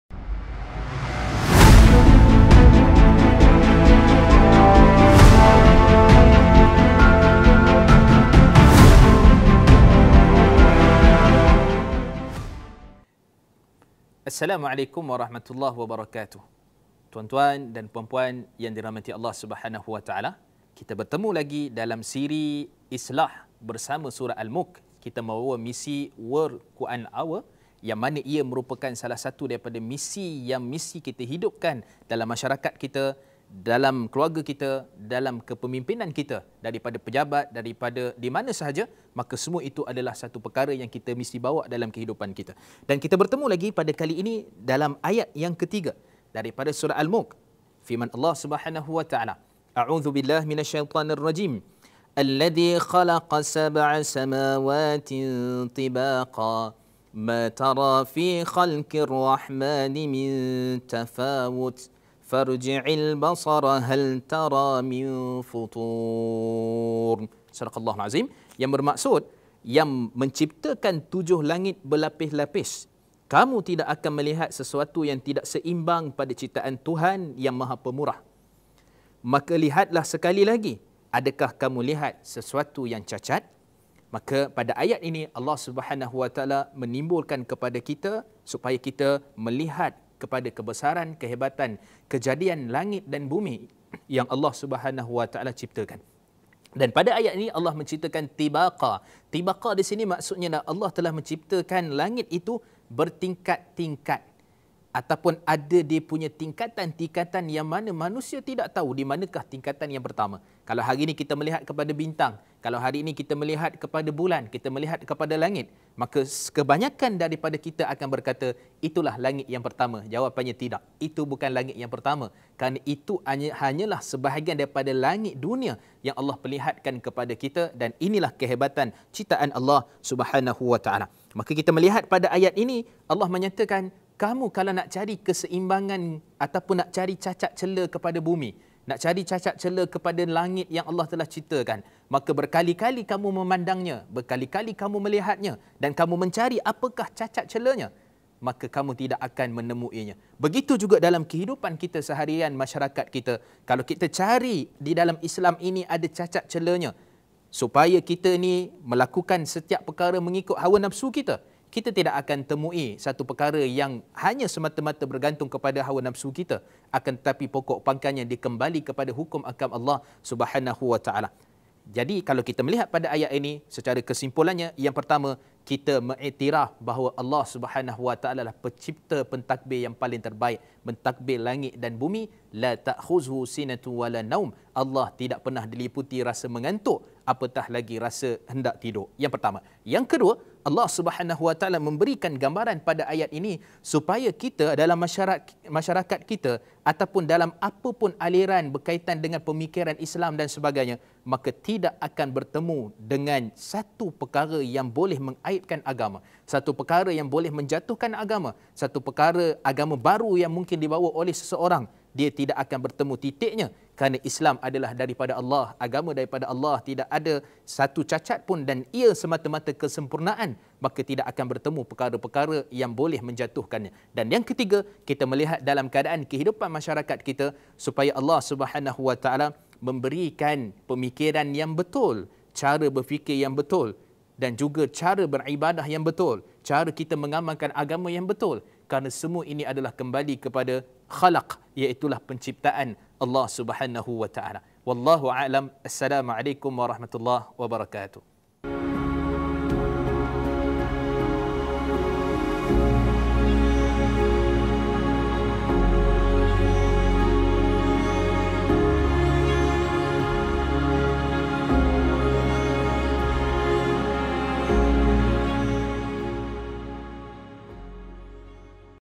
ISLAH BERSAMA AL-MULK, AYAT 3
Segala yang Allah SWT ciptakan adalah penuh kesempurnaan. Tiada sebarang kecacatan atau kekurangan pada ciptaan-Nya termasuklah penciptaan kita.

Penciptaan manusia tersangatlah abstrak. Oleh itu, kejadian kita ini mempunyai nilai yang sangat tinggi di sisi Allah SWT. Jika kita mula rasa ada keraguan, pandanglah ke langit.

Perhatikan penciptaan tujuh lapisan langit dengan sangat teliti, seimbang dan sempurna. Begitu jugalah segala kejadian lain di alam semesta ini. Dan pastinya kita tidak akan berupaya menemui sebarang kecacatan atau kekurangan pada ciptaan-Nya.

Justeru, kita sebagai hamba-Nya hendaklah mengikut segala yang diperintahkan oleh Allah SWT dalam segenap aspek kehidupan kerana segala yang datang dari-Nya, termasuk syariat yang diturunkan adalah sangat sempurna dan sesuai untuk kemaslahatan hidup manusia.

Ketahui lebih lanjut tentang motivasi melalui ayat 3 Surah al-Mulk ini bersama sahabah al-Quran kita, Ustaz Mustafar Mohd Suki.